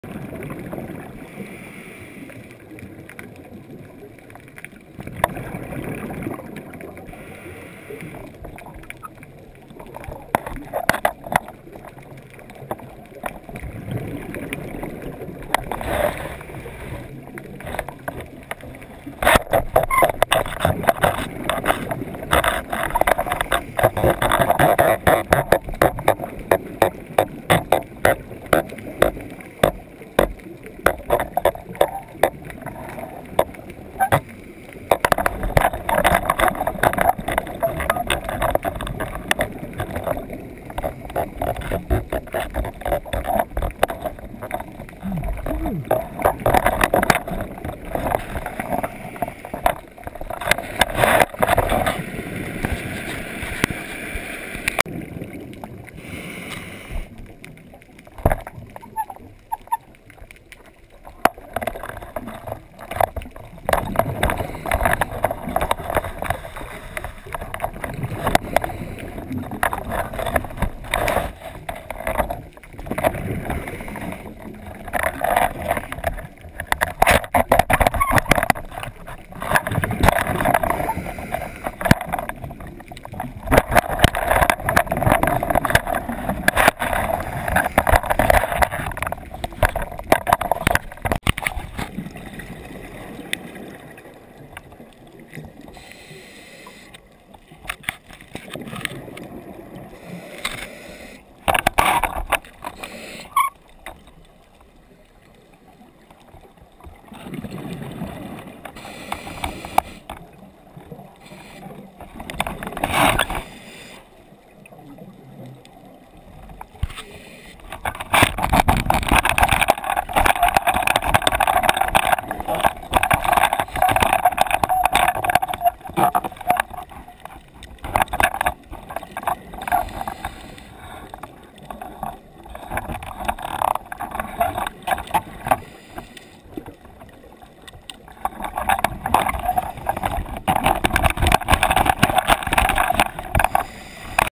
How to Snare a Lobster
Short video on using a snare to catch a Florida lobster (spiny lobster or crawfish). Video captured using a GoPro Hero 2 attached underneath a lobster snare.